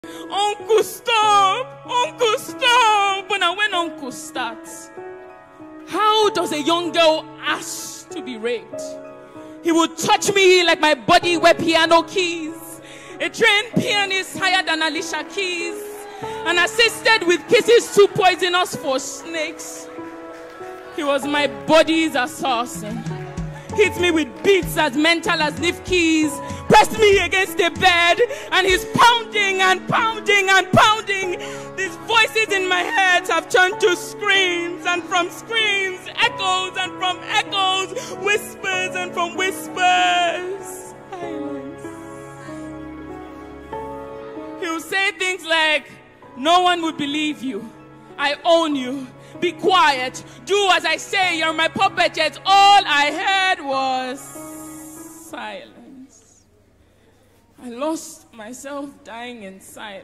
No one ever deserves to go through this 🥺 Say No to R*pe ❌
Ibquake & boneless definitely delivered a great message in this performance🔥🔥